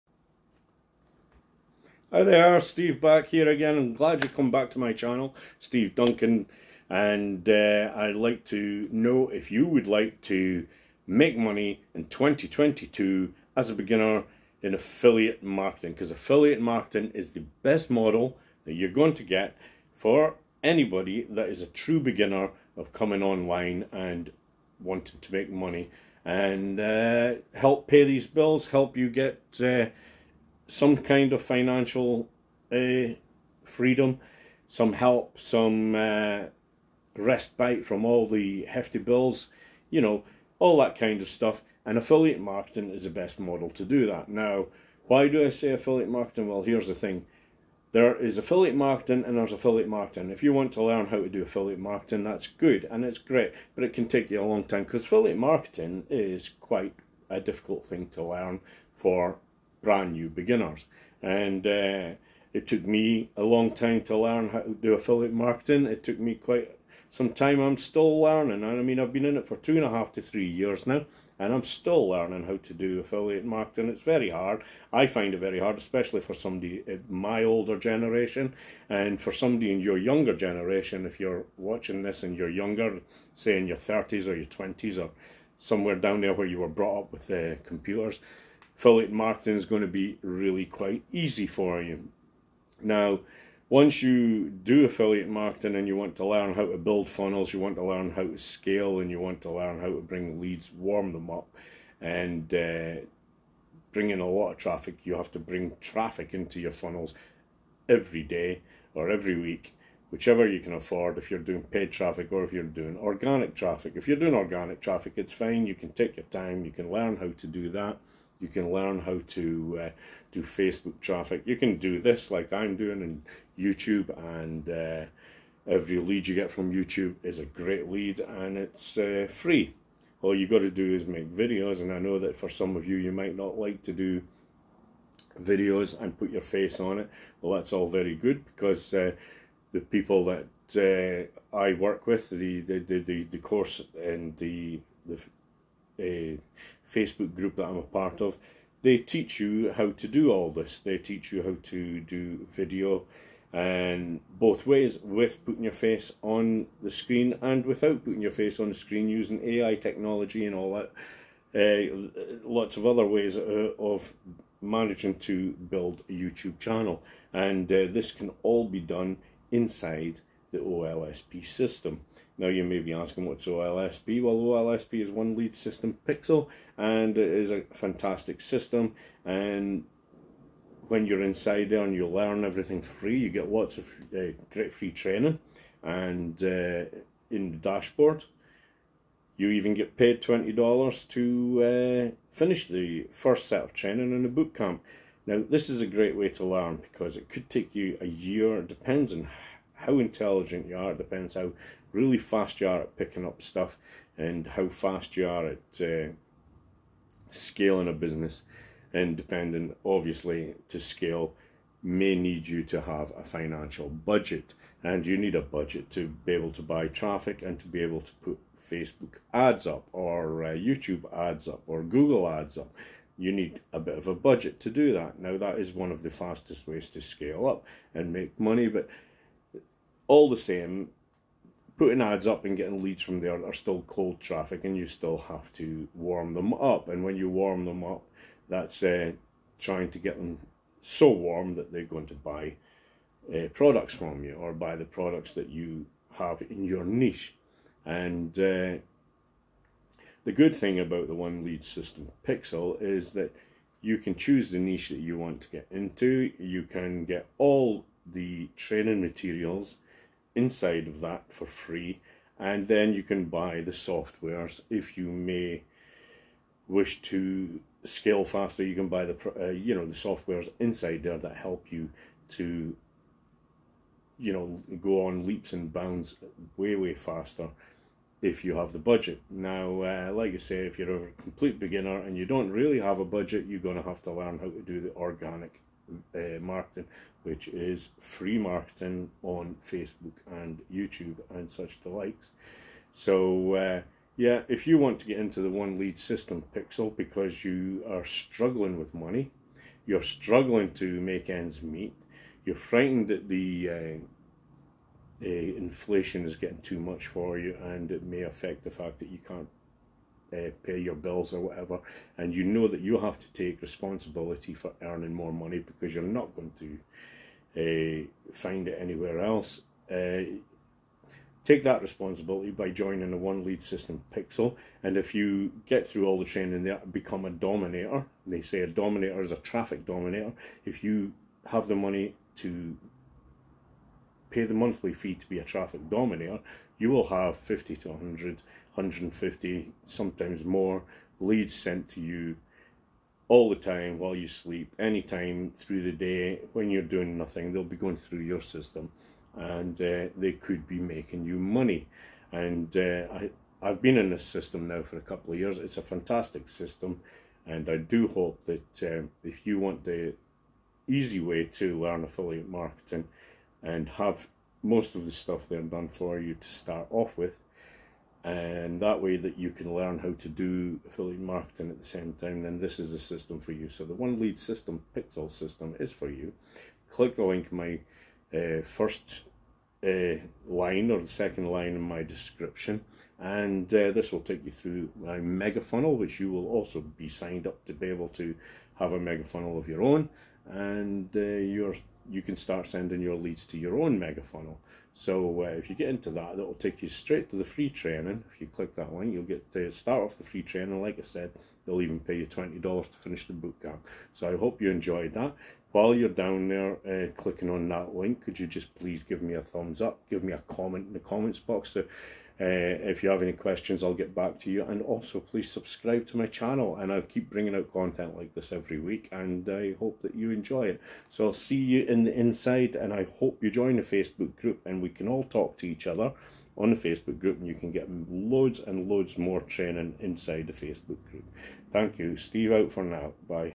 How to start Affiliate marketing as a complete beginner in 2022
How to get started in affiliate marketing is the best way for a complete beginner in 2022 and earn while you learn.
AFFILIATE Link source I use most